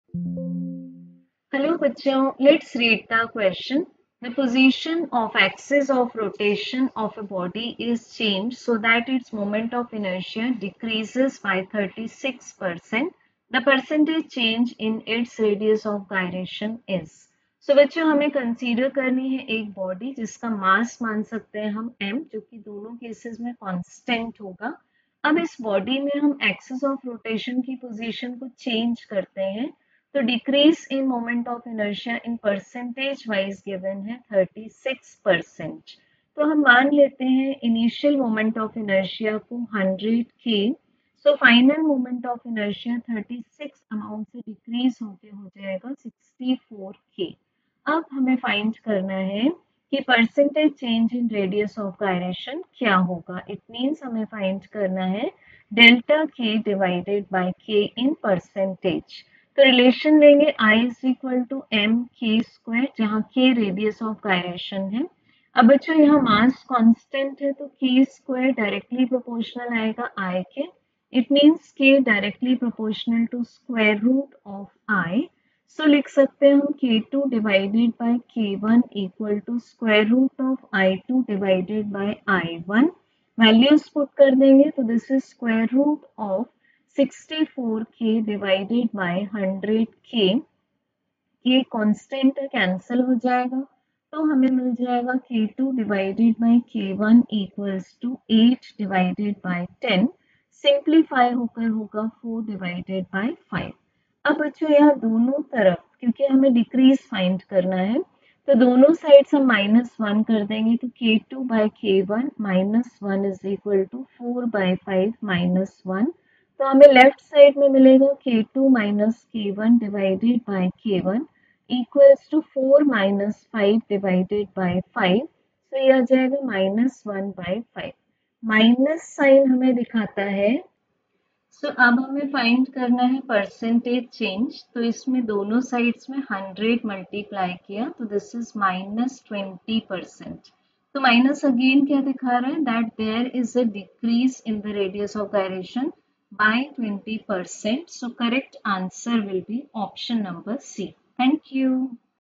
The position of axis of rotation of a body is changed so that its moment of inertia decreases by....
The position of axis of rotation of a body is changed so that its moment of inertia decreases by 36%. The % change in its radius of gyration is.. 📲PW App